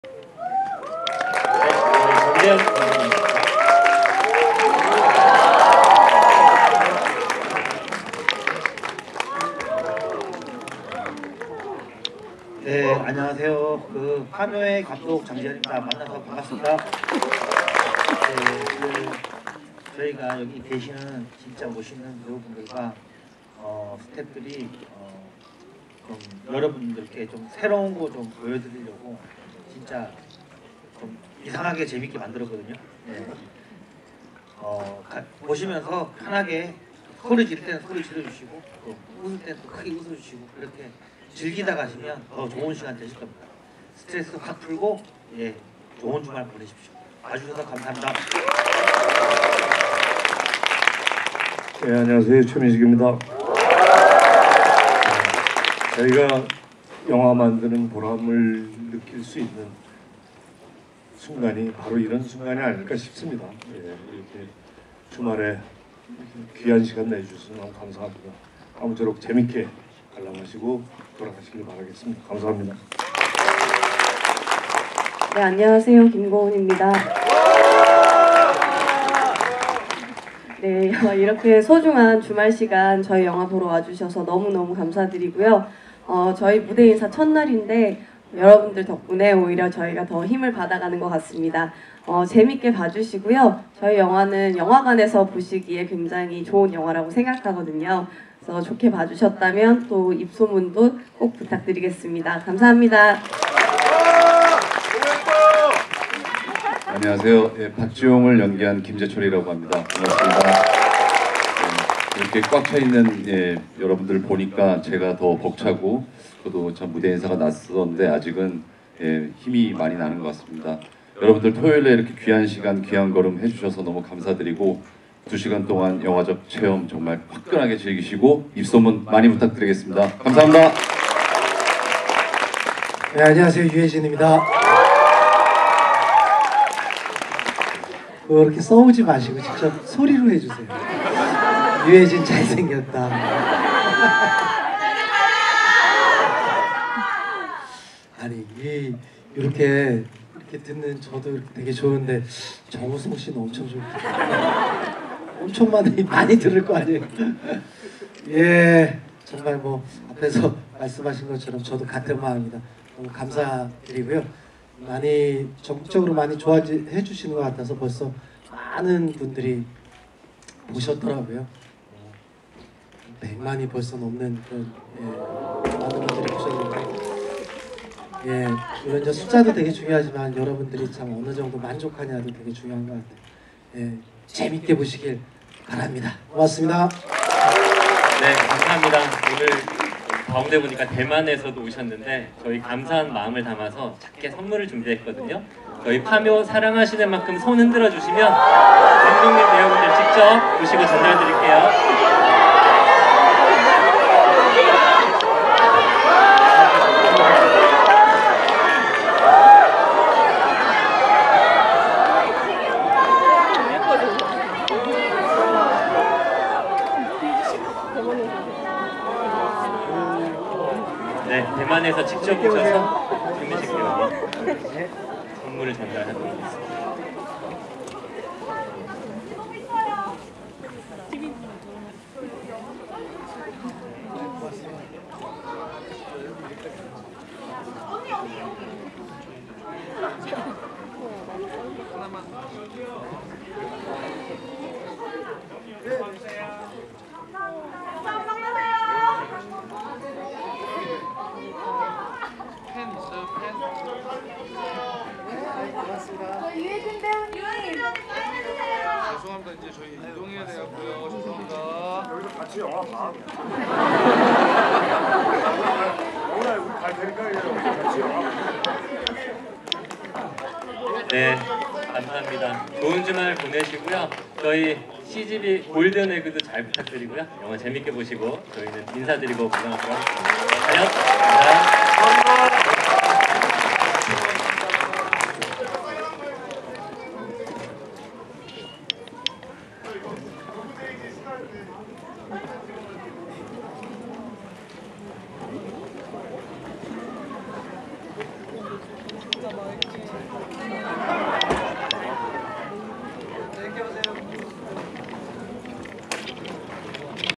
영화 '파묘' 무대인사 (최민식, 김고은, 유해진, 장재현 감독) 240224
파묘 무대인사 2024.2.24. / CGV 여의도 3관 15시 시영 무대인사

참석 : 최민식, 김고은, 유해진, 장재현 감독